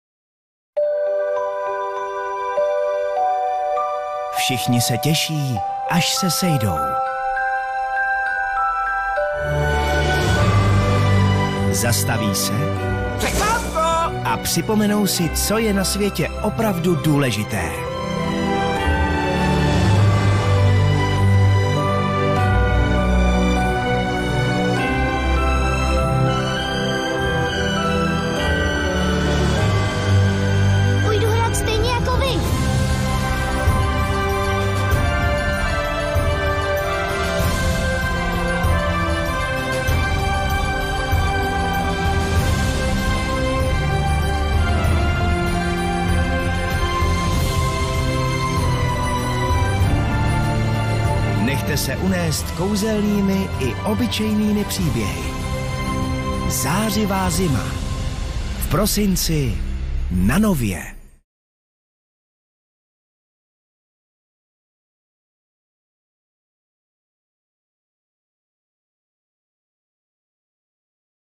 Zářivá zima na Nově - Vánoce 2019
Upoutávka na filmy a pohádky během Zářivé zimy na televizi Nova - Vánoce 2019.